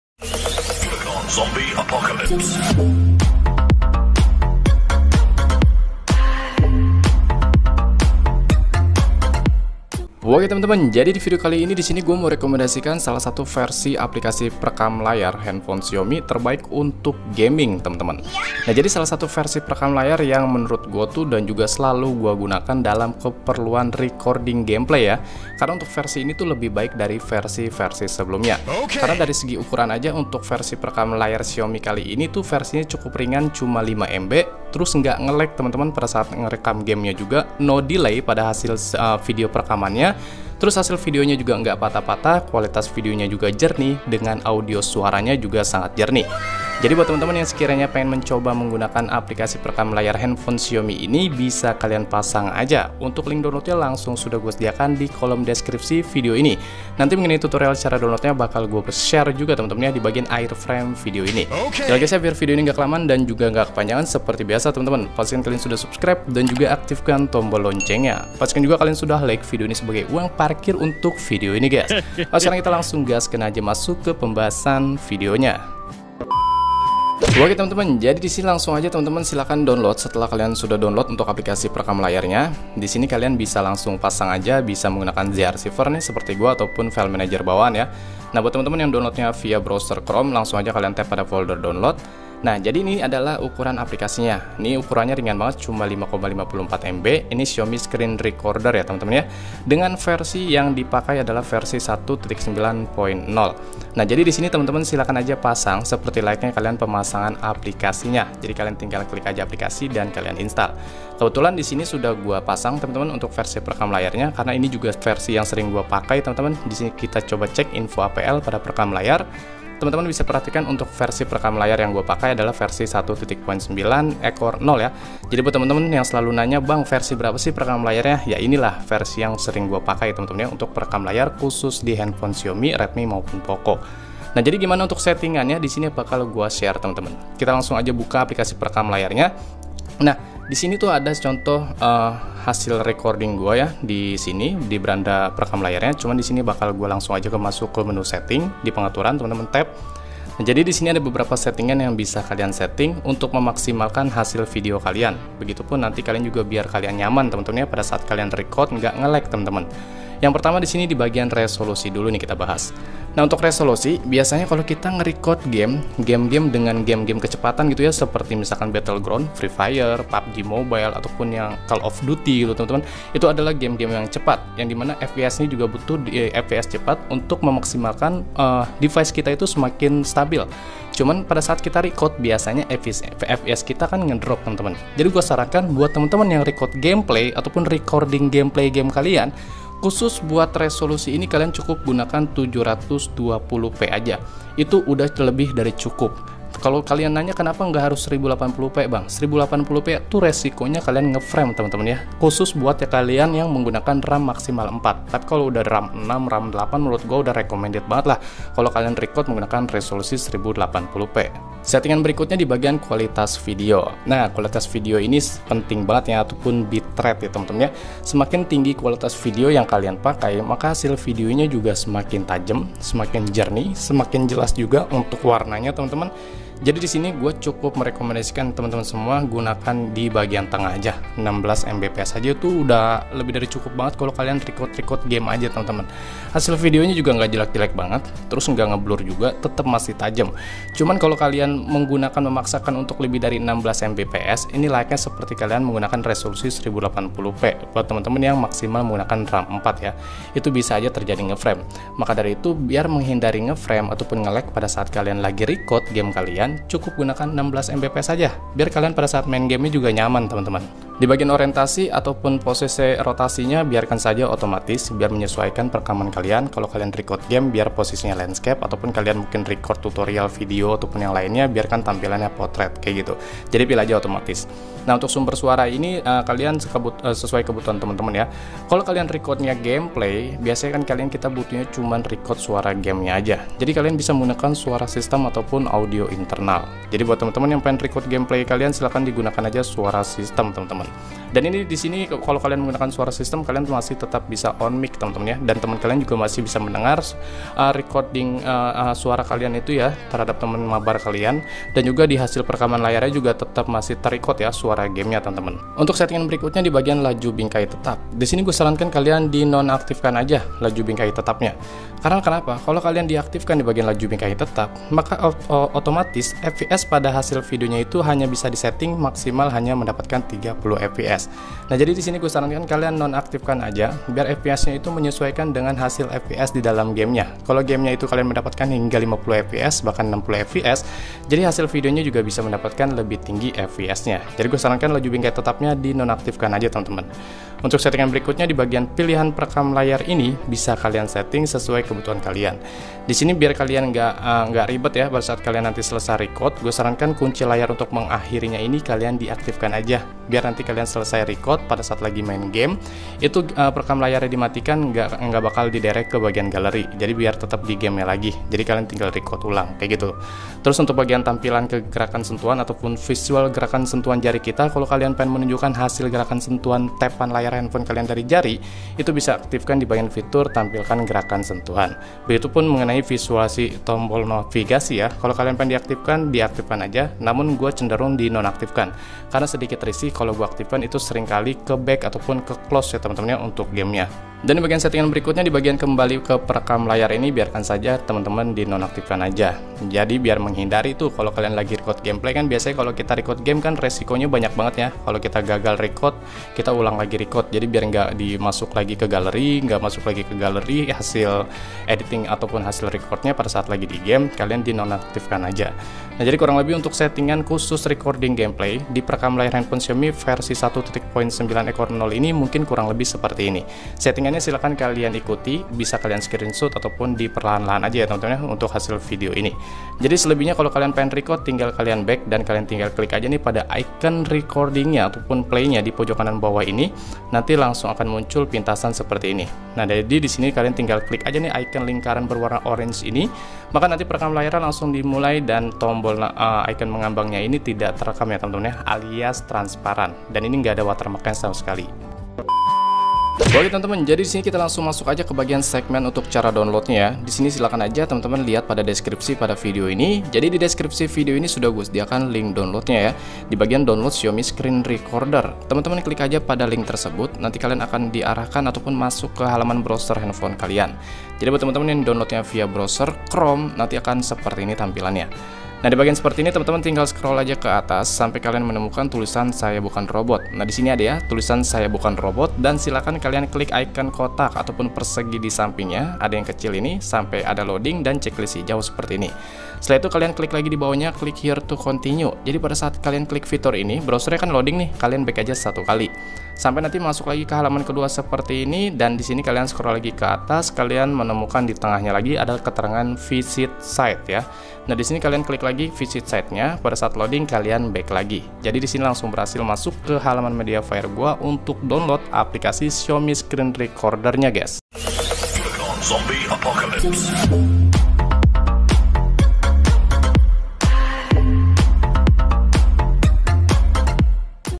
APK PEREKAM LAYAR HP TERBAIK UNTUK GAME‼️MIUI SCREEN RECORDER
Guysss...
Video kali kali ini saya ingin merekomendasikan salah satu Versi Apk perekam layar hp Xiaomi terbaik untuk Game. MANTAP!

Salah satu Versi perekam layar Hp yang selalu saya gunakan dalam keperluan Recording Gameplay. Karena untuk Versi ini lebih baik dari Versi lainnya.
✅ Versi lebih Ringan size 5Mb
✅ No lag saat record game
✅ No delay
✅ Hasil Video tidak patah-patah
✅ Kualitas Video jernih
✅ Audio Jernih

(Cara Download dalam Video)

Buat kalian yang mau membantu channel
Raka Anggara, bisa Donasi kesini :